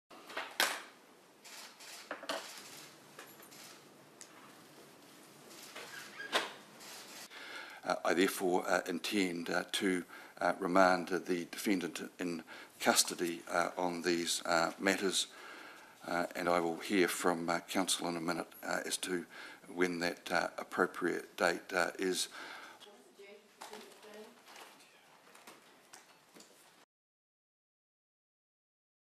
44-åring anklagas för att ha spritt video från Christchurchdåden
44-årige Philips Arps anklagas för att ha spritt videon som terroristen i Nya Zeeland livesände under moskéattackerna.
Bildmaterial: AP